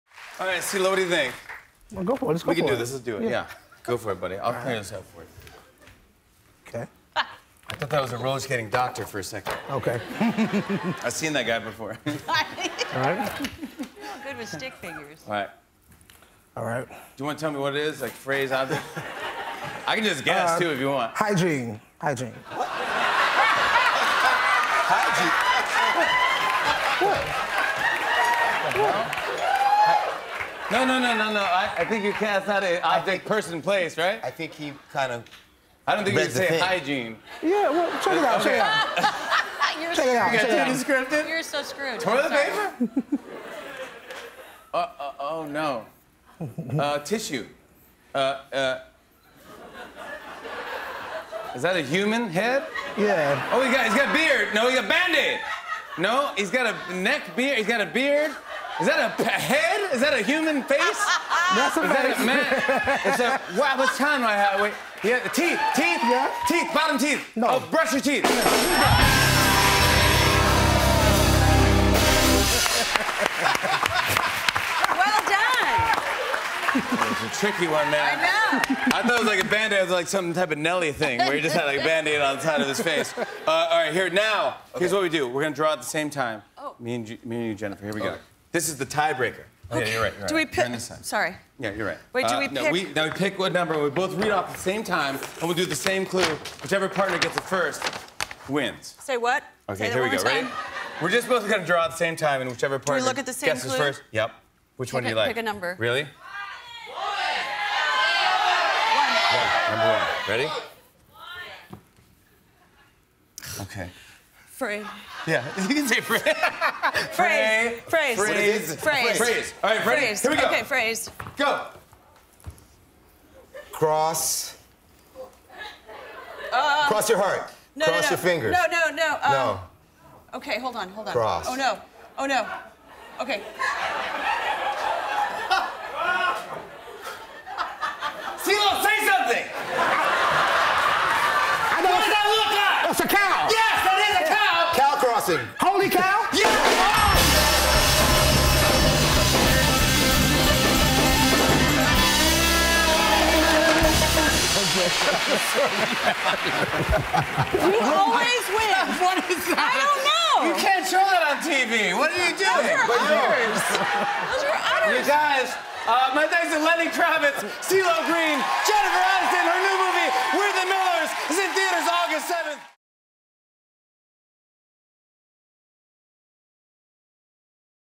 Pictionary with Jennifer Aniston, Lenny Kravitz and CeeLo Green, Part 2
Jimmy teams up with CeeLo Green to play a game of Pictionary against Jennifer Aniston and Lenny Kravitz. Part 2 of 2